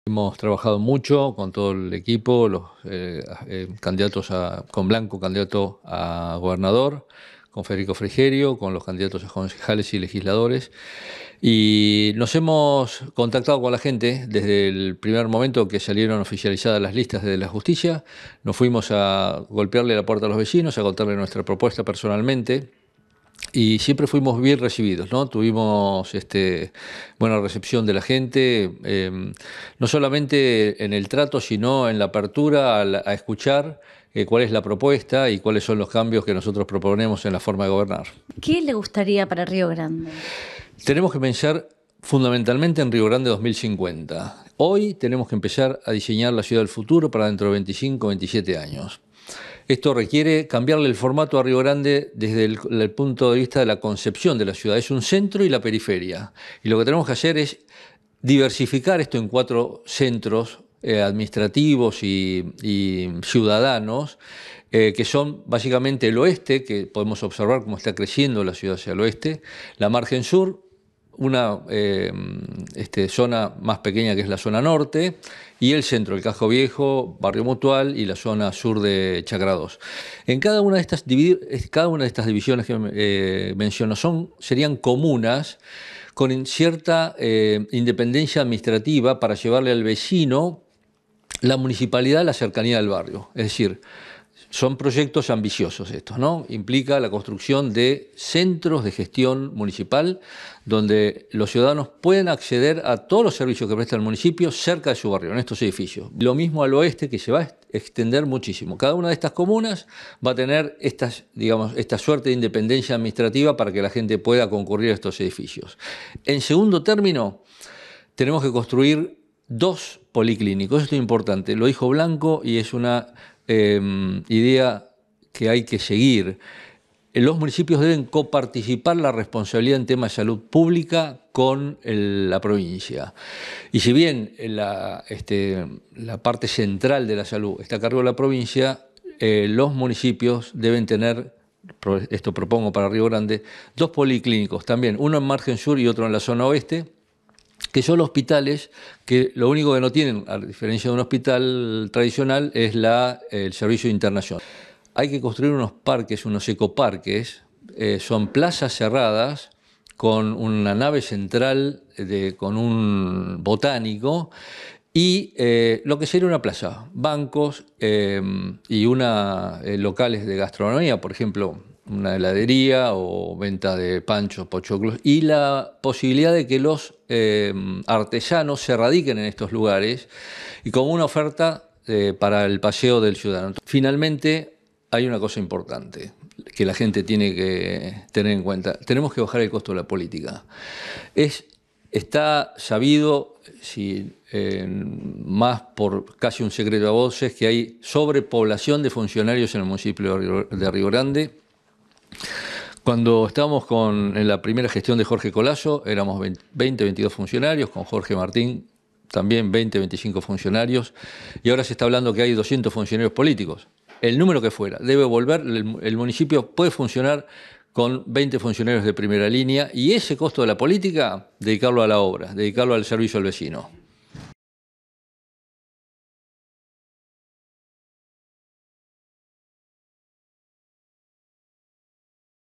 RÍO GRANDE: CANDIDATO PROPONE MÁS AUSTERIDAD
Dos veces fue secretario de finanzas en el municipio. El contador Fernando Bacichetto, candidato de Juntos por el cambio asegura: “todo debería marchar bien con 20 funcionarios”.